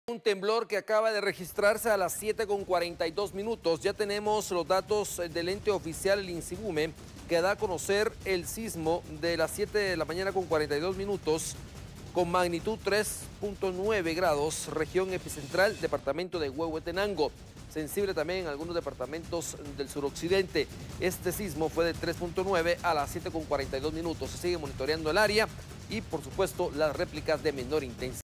Sismo de magnitud 3.9 en Guatemala
Autoridades monitorean la situación

El INSIVUMEH reportó un sismo de magnitud 3.9 con epicentro en el departamento de Huehuetenango. Visita nuestro sitio